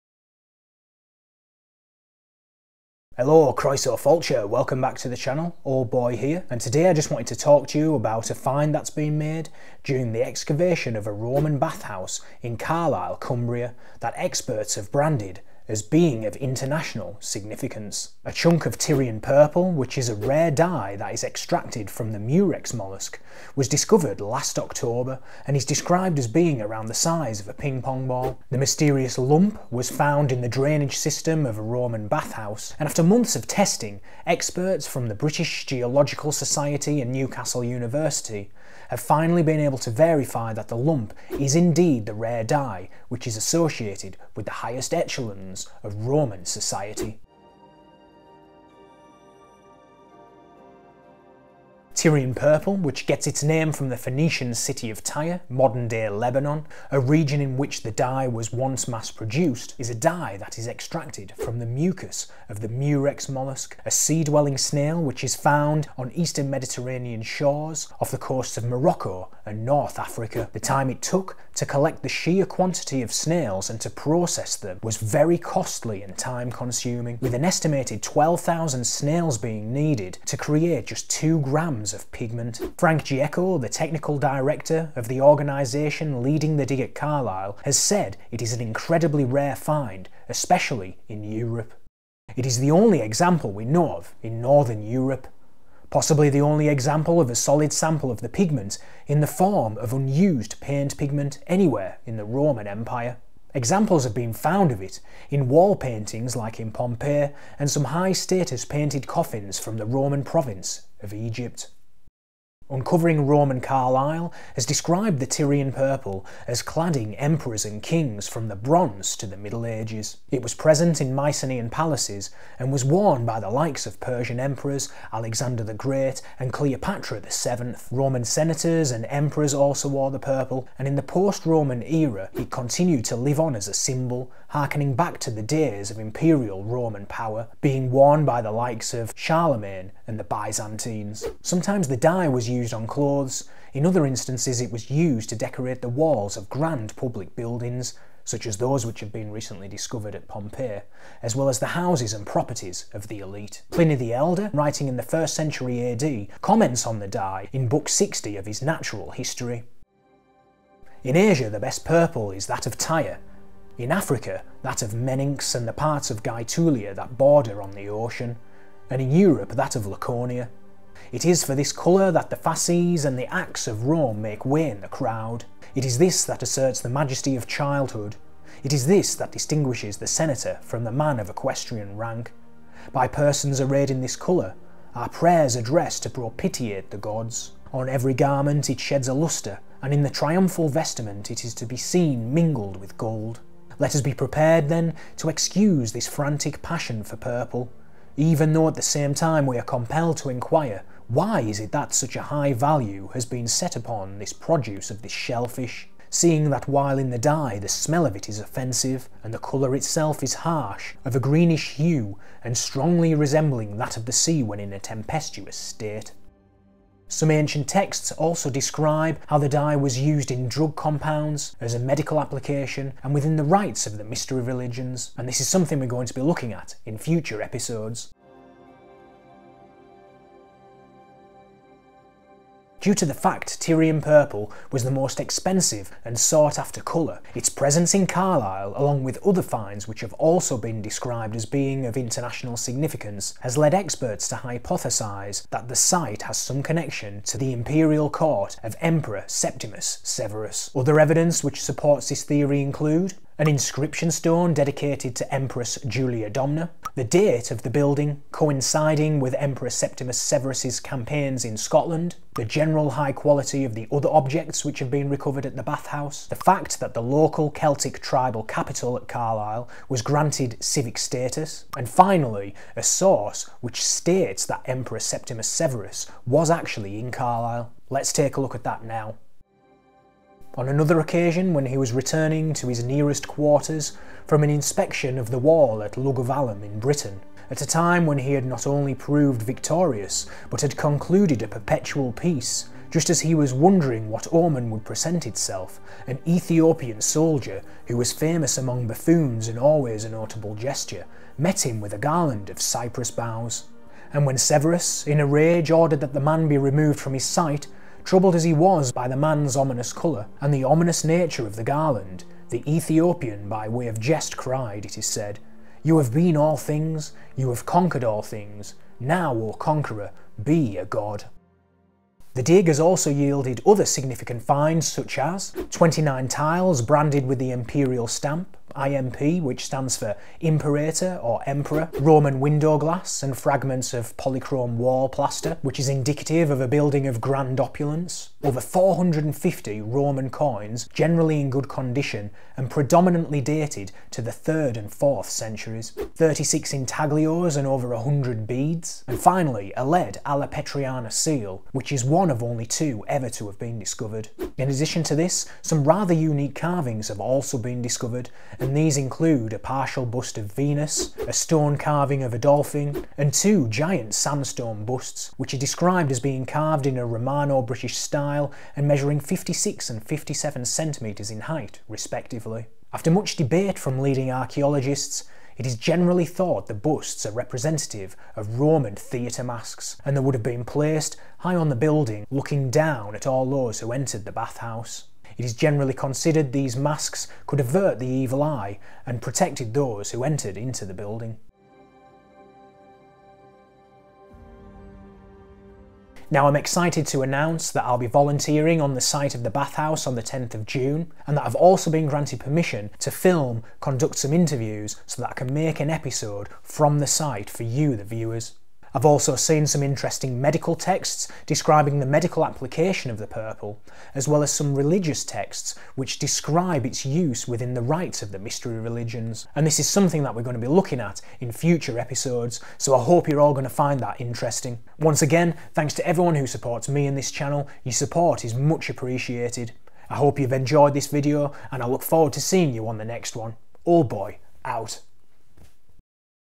Tyrian Purple Found in Roman Bath House (Carlisle, England - 2023 Discovery)
In October 2023, archaeologists unearthed an extraordinary discovery while excavating a Roman Bath House in Carlisle, Northern England—a lump of unused Tyrian Purple dye. After months of testing, experts from Newcastle University confirmed that the dye was Tyrian Purple, a rare and valuable pigment used by the elite in Roman society.

Tyrian Purple, made from the mucus of the Murex mollusc, was a costly dye coveted for its deep hue. The process to create just a small amount was labor-intensive, requiring over 12,000 snails to produce only a gram or two of dye. This dye was so precious that it was worth more than gold and symbolized power and prestige.

The discovery in Carlisle is being hailed as a significant find, potentially linked to the Imperial Court of Emperor Septimius Severus, based on the high-quality artifacts uncovered at the site. This video explores the importance of this find and what it reveals about Roman Britain.

In this video, we explore:
💜 The discovery of Tyrian Purple in Carlisle
🐌 How Tyrian Purple was made from the Murex mollusc
🏛 Connections to Emperor Septimius Severus and the Roman elite
📜 The significance of the find for Roman Britain

Join us as we uncover the fascinating story behind this rare discovery in the heart of Roman Britain!